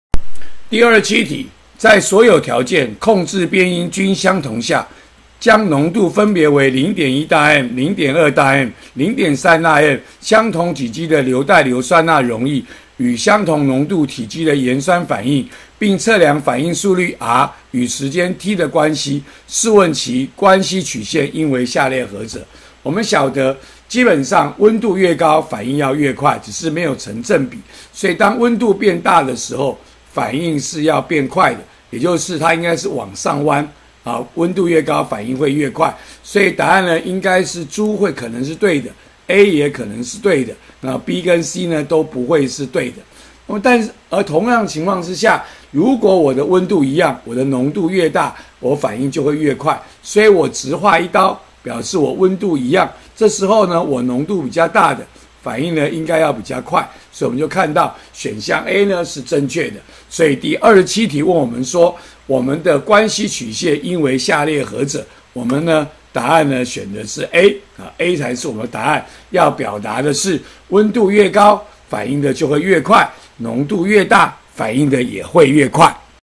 103學年下8年級理化二段第27題
問不同濃度、不同溫度，反應速率與溫度關係圖為何？